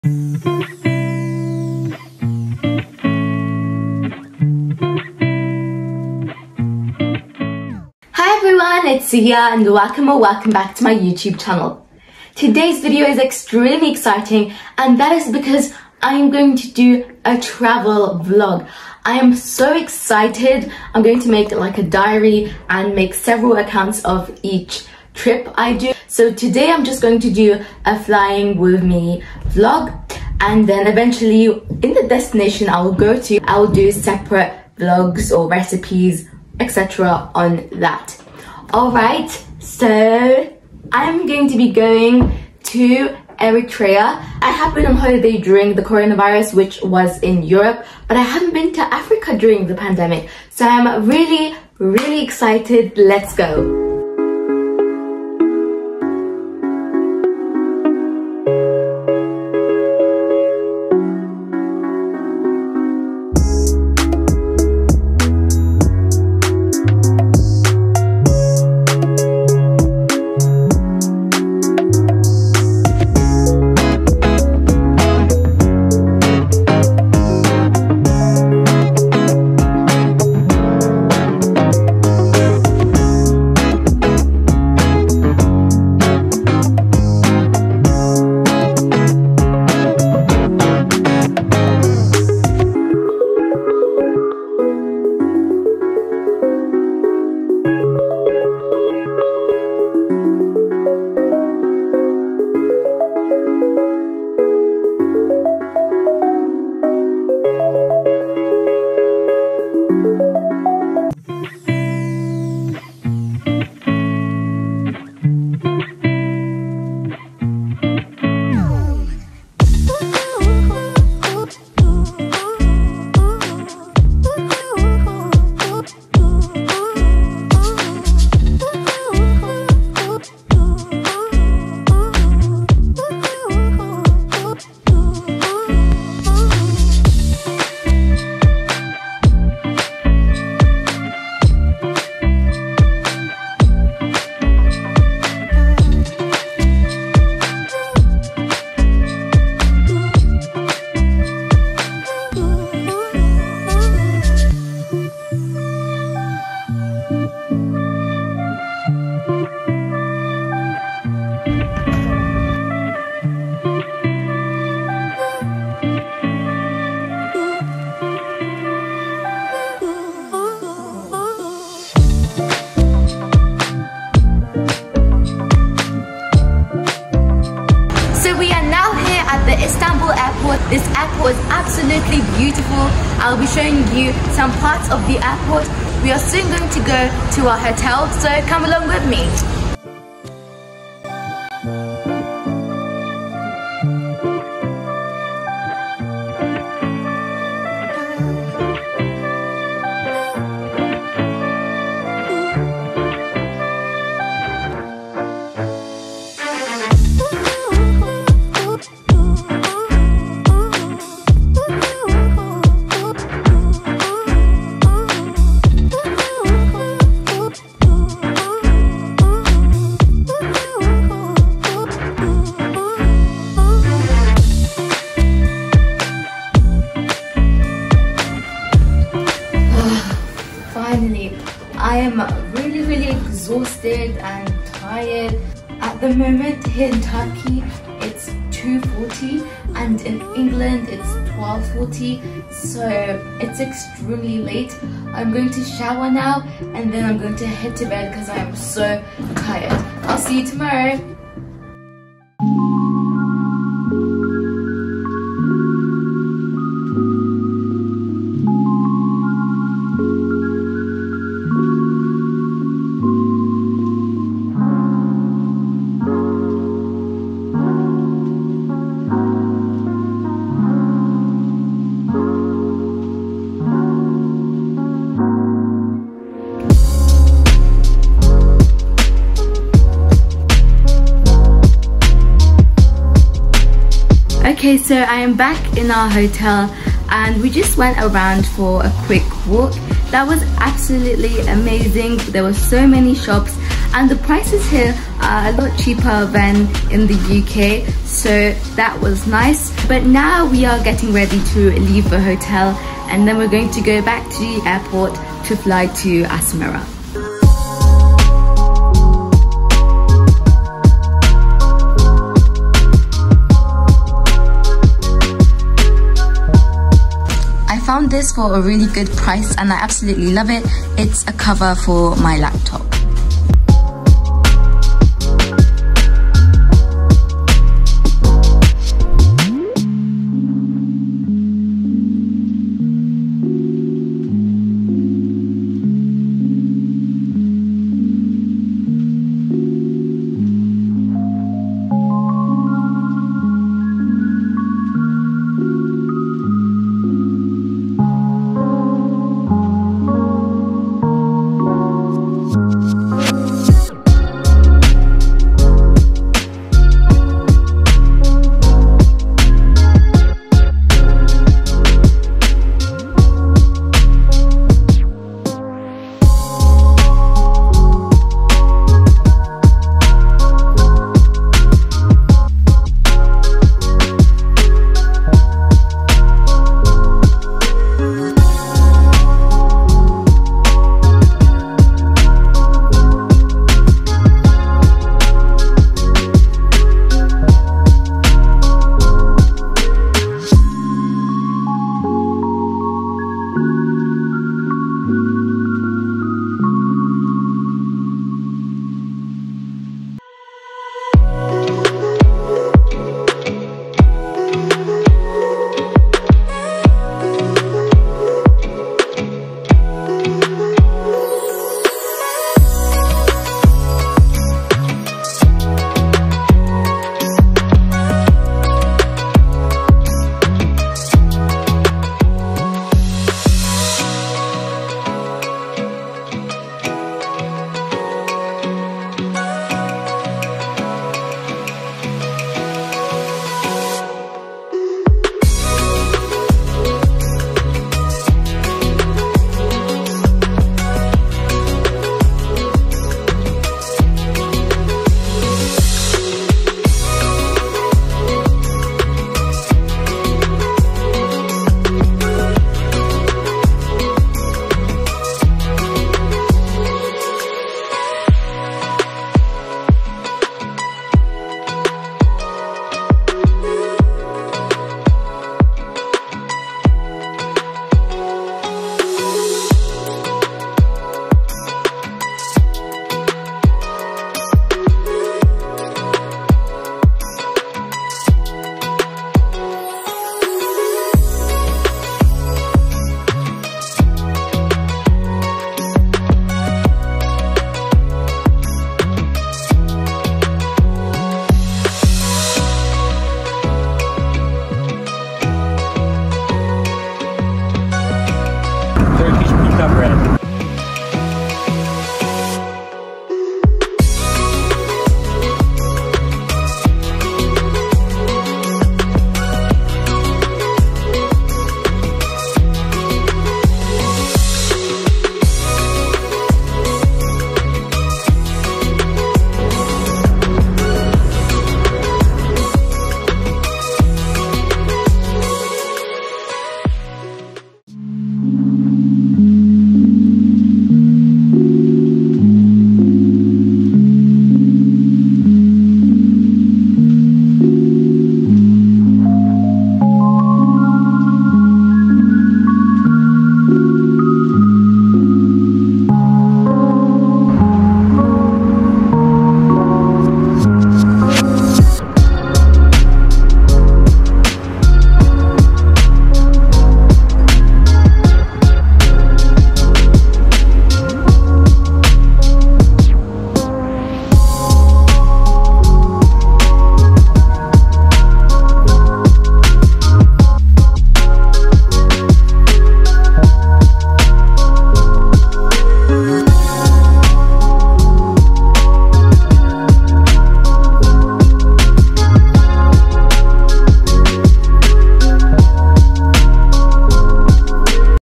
ERITREA TRAVEL VLOG ✈️: Travel with me to ASMARA (airport, hotels & food! 🌯)
Travelling to ASMARA ☀️for the first time during the C-19 pandemic! In this video, I show my long journey to Asmara, Eritrea featuring airports, aeroplanes, food and hotels 🏨 I hope you will enjoy watching this video & stay tuned for more videos in my ERITREA TRAVEL VLOG series!

Second Channel: Cia B Shorts

(Downloaded on CapCut)